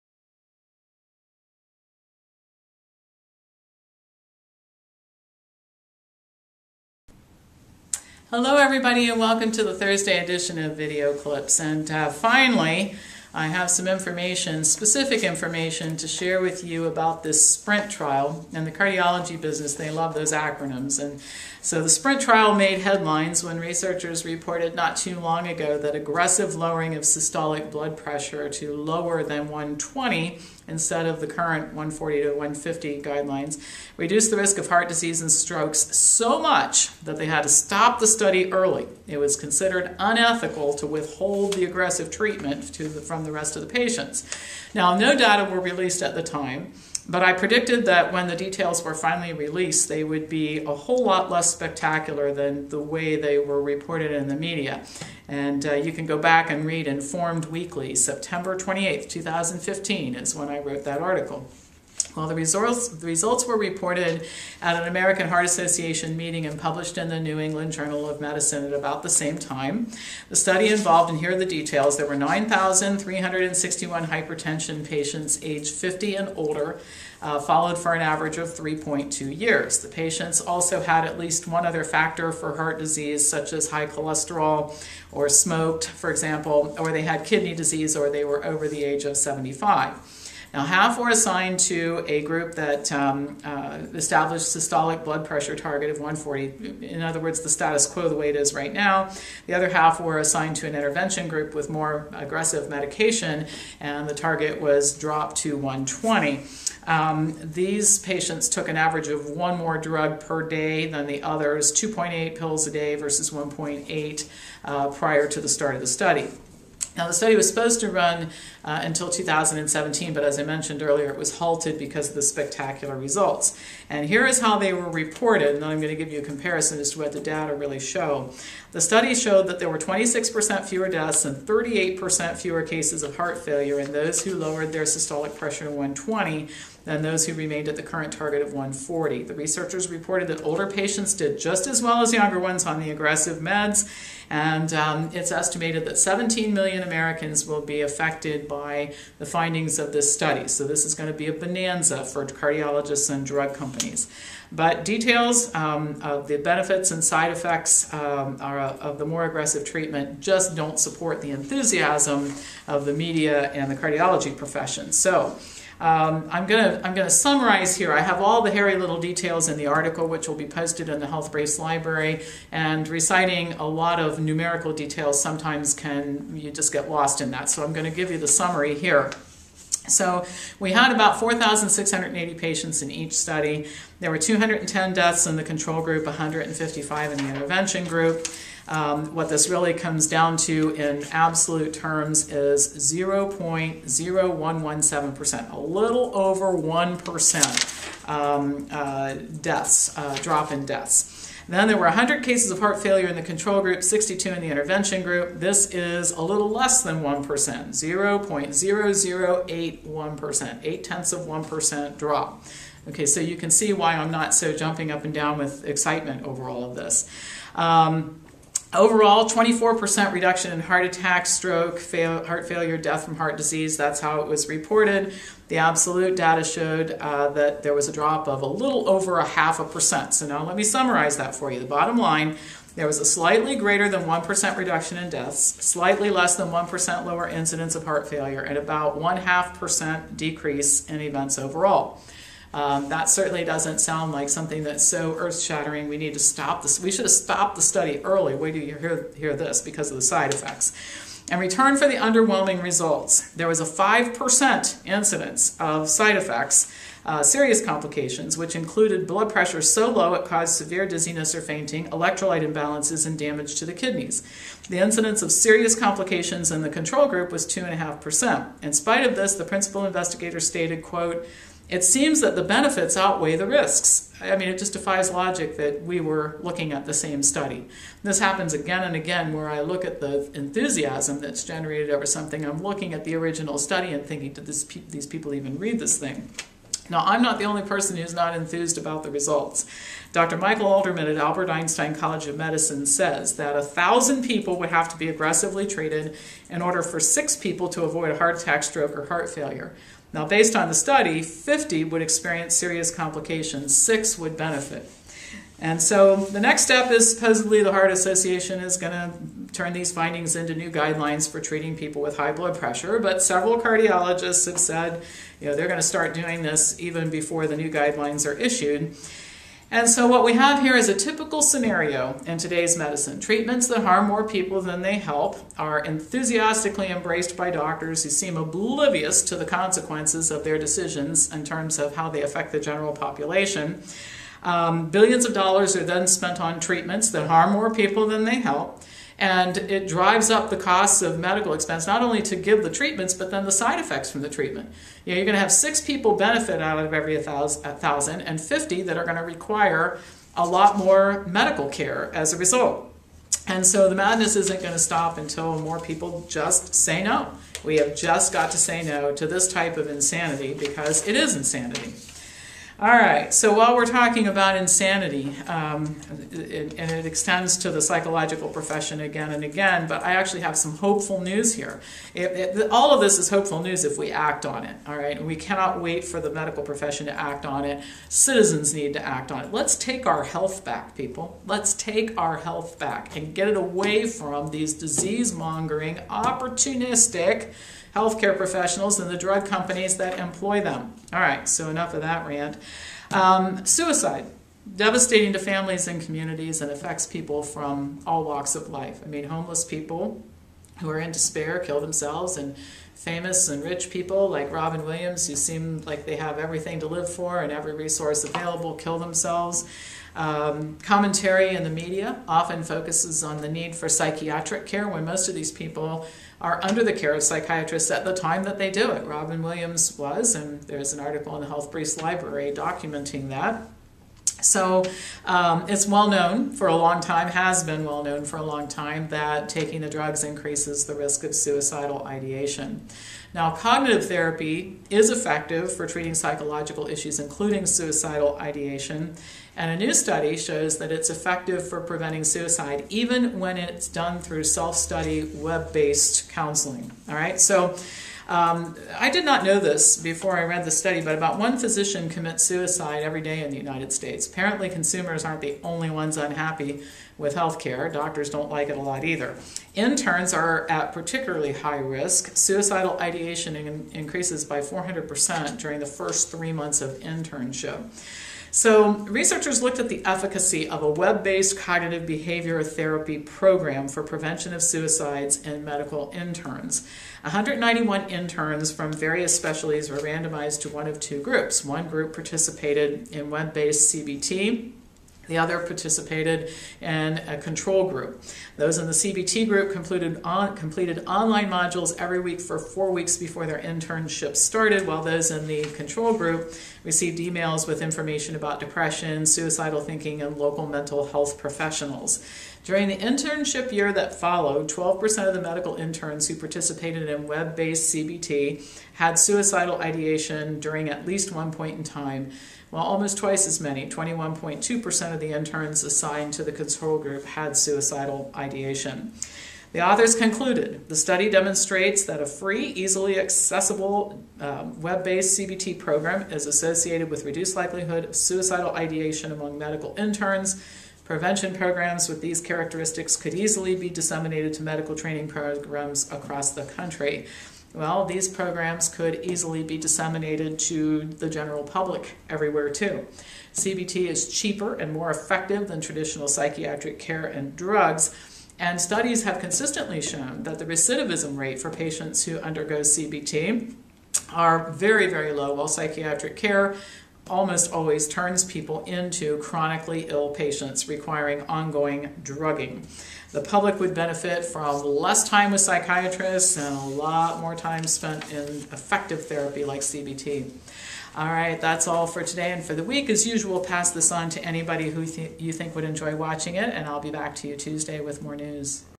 Analysis of the SPRINT Trial AND WebBased Cognitive Therapy Effective for Preventing Suicide
The headlines reported that aggressive medication to lower systolic blood pressure even more was so successful that the trial had to end early, but the results as recently reported do not support that claim.

Cognitive Behavioral Therapy is so effective that even when delivered via four web-based self-study sessions, it significantly reduced suicidal ideation for interns.